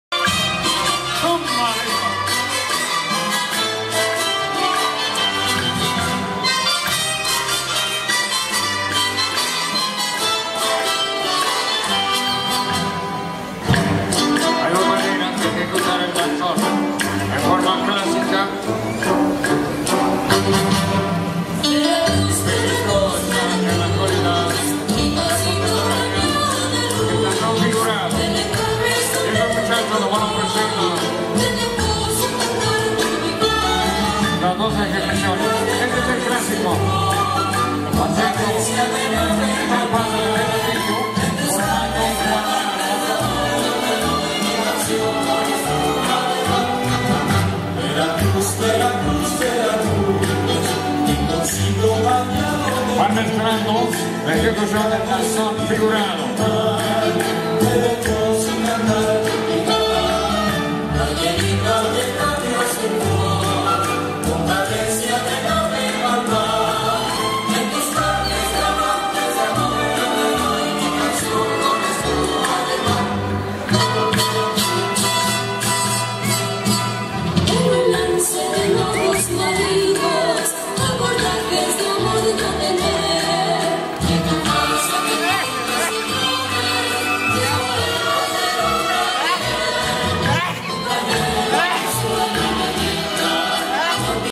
Qué bello es VERACRUZ
In Mérida, Yucatán at the beginning of my month long trip to the MAYA regions of MEXICO and GUATEMALA. Sunday, June 9th 2013.